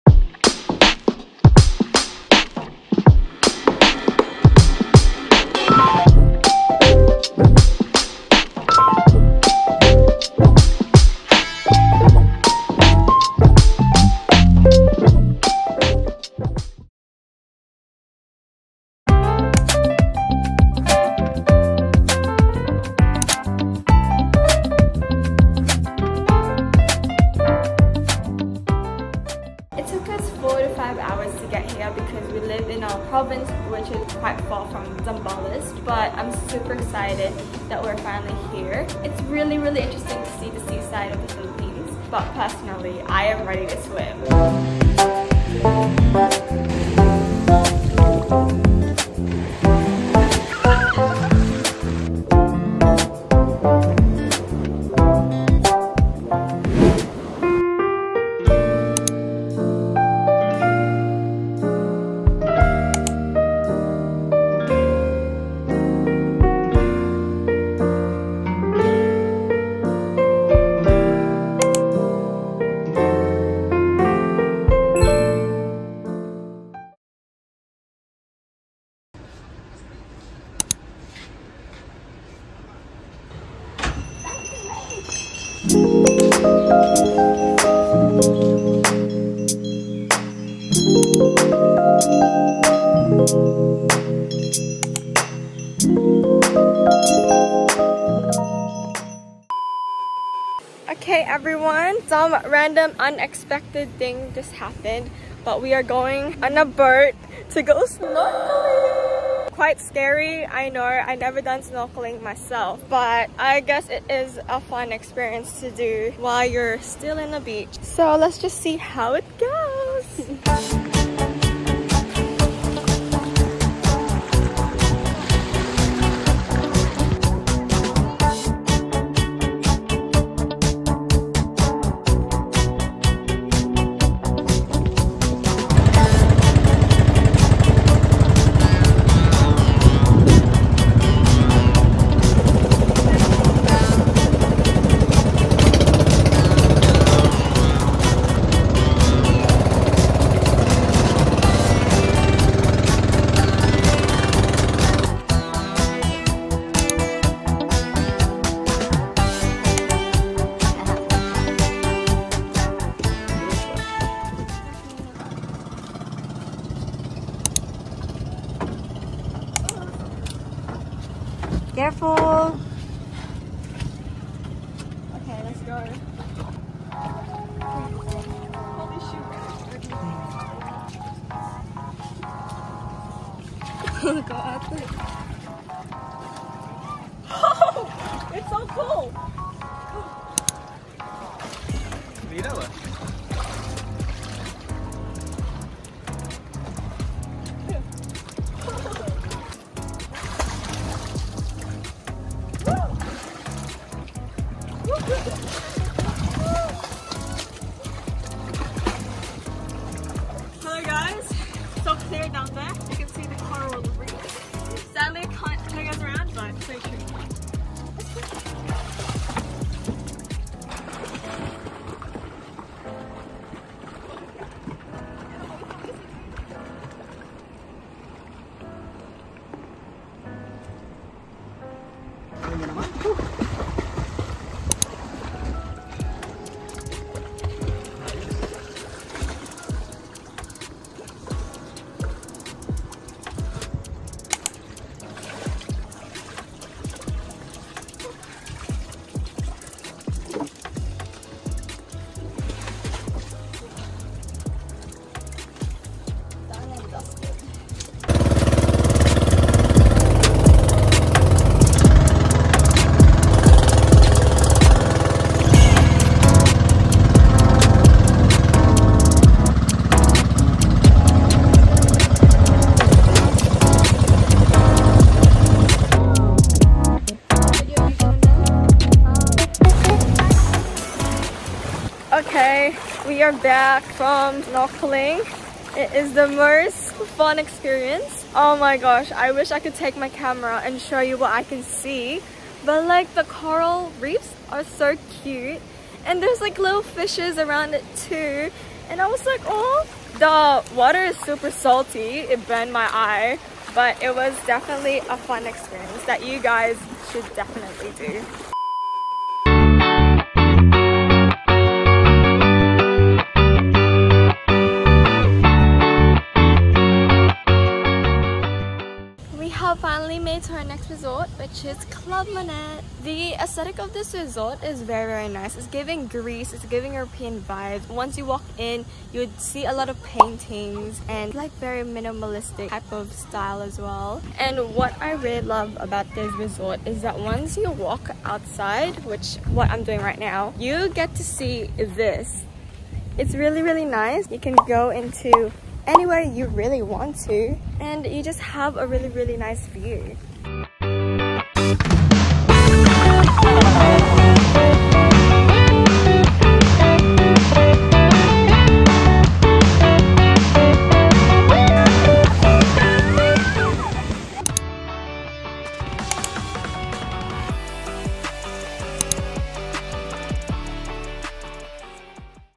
beach vlog🏝️ | resorts, beaches, snorkeling🤿, delicious food, and more!!
ck’s note: watch in HD!!🌷
— hello, everyone! welcome to newest video, a beach vlog in ZAMBALES‼️
— watch me experience snorkeling for the first time, try out delicious food, and showcase fantasy resorts in Zambales :))

for those who are new to my channel, i just want to say “hiiii😙🤍” — my name is ck :) and i make vlogs for fun on a daily basis!!

🏝️FUNTASEA HOTEL & BEACH RESORT🏝️
📍Location/Address: Purok 5 Special, Iba, Zambales

Entering Funtasea, the resort welcomes guests with friendly and energetic staff members. As soon as you get inside the resort, you will see the main lobby, two swimming pools (one for children, and the other for adults), and the amazing view of the beach. There’s multiple restrooms and shower areas for guests to clean up before entering into a building.
There is a restaurant and a bar for guests to eat and drink, as well as an acoustic performance by a lovely singer during the night! :)

Once you checked in, you’ll receive a wristband that you must wear 24/7 on the resort.
— The rooms are spacious and comfortable, and the reception provides you with a “guest kit” that comes with a soap, toothbrush, room slippers, and shower cap.

‼️Quick Q&A‼️
🌱Is there free wifi? Yes!! Funtasea has incredible wifi service— it’s so strong that you can watch YouTube videos on the beach!!
🌱Do they provide free breakfast/meals? It depends on the package. But on my experience, and based on what my parents paid for, we received free breakfast. But don’t worry, if your package doesn’t receive any free breakfast/meals, you could always buy meals in the restaurant/bar.
🌱Will there be activities to do? It depends on when you’re staying in the resort. My family and I were lucky enough to experience snorkeling, but from what we heard from staff members, they would have banana boat rides on April since the sea is much calmer during that month :))

🏝️CLUB MONET BEACHFRONT RESORT (by Cocotel)🏝️
📍Location/Address: Felmida-Diaz, Cabangan, Zambales

As mentioned in the video, the aesthetic of this resort is definitely giving Greece/European vibes. The style of resort is absolutely breathtaking and guest have lots of opportunities for “instagramable” photos. Walking up to the reception, you will be greeted by a lovely dog and friendly staff members.

There is a restaurant where you can order food, but you must inform the staff members 2 hours prior eating time.
I didn’t spot any bars, but there are plenty of places where you can chill and relax on the beach.

Unfortunately, we didn’t get to swim in the beach since the waves were super strong.
— There is a pool, but it is mainly targeted for children ages 6-12 years old :))

‼️Quick Q&A‼️
🌱Is there free wifi? There is free wifi, but you will need to ask the staff members for the password.
🌱Do they provide free breakfast/meals? Once again, it really depends on the package. But don’t worry, if your package doesn’t receive any free breakfast/meals, you could always buy meals in their restaurant (but please note: unlike with Funtasea, you must inform the staff members 2 hours prior before you want to eat‼️)
🌱Will there be activities to do? From our stay in the resort, sadly, we haven’t heard anything about activities such as boat riding or banana boat.

ck’s note: when going on beaches, I recommend wearing LOTS OF sunscreen, wear your cute sunglasses, and bring a fan since it can get hot‼️
— bring lots of spare change of clothes and a camera to capture memories :D

until then, see you all in my next video!
bye bye :)